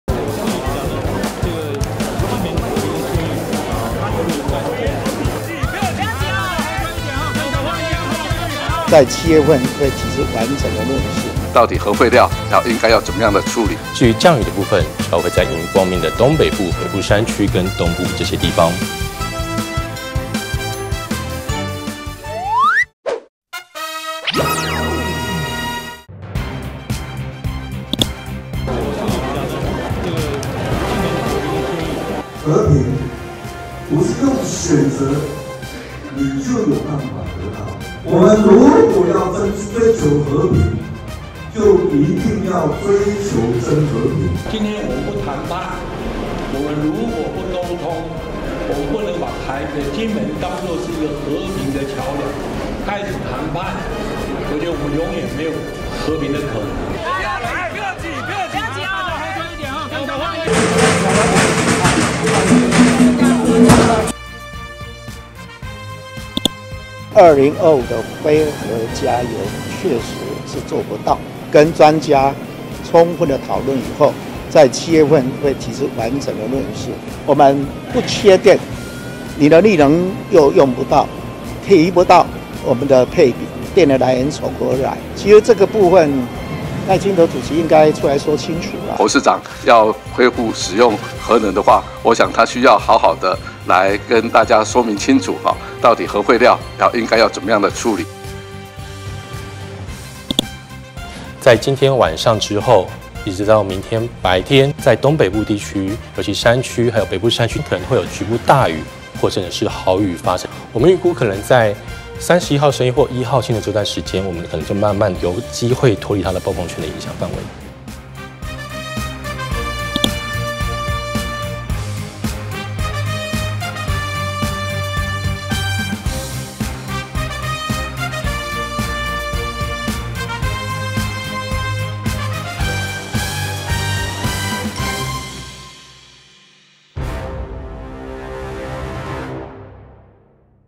【0530今日HOT新聞】在金門「同框」！郭、賴交流和平；郭、柯一起抬轎｜侯：7月提完整核能論述 陳揆：核廢料怎處理？｜「瑪娃」挾豪雨 最快5/31深夜有望脫離暴風圈
今天HOT新聞有哪些？
今天最政：在金門「同框」！郭、賴交流和平；郭、柯一起抬轎
今天最熱：侯：7月提完整核能論述 陳揆：核廢料怎處理？
今天最猛：「瑪娃」挾豪雨 最快5/31深夜有望脫離暴風圈
今天最樂：台積電首季業績獎金開獎 員工爆料較去年第4季「打7折」